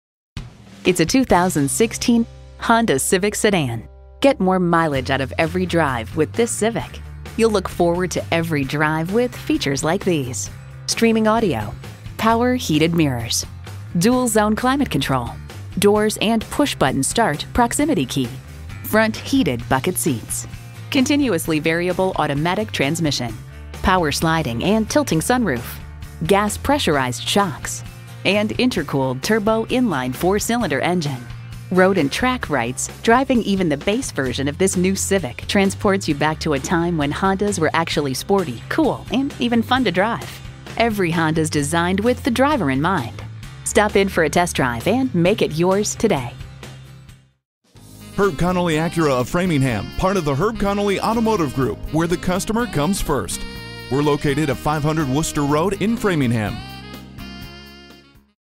Used 2016 Honda Civic Framingham Natick Marlborough MA, MA A6159P - SOLD
Year: 2016
Make: Honda
Model: Civic
Trim: EX-T
Engine: 1.5L I-4 DI DOHC Turbocharged
Transmission: CVT
Color: Black
Interior: Black/Gray
Mileage: 23033
Stock #: A6159P
VIN: 19XFC1F37GE216714

Address:
500 Worcester Road Route 9 E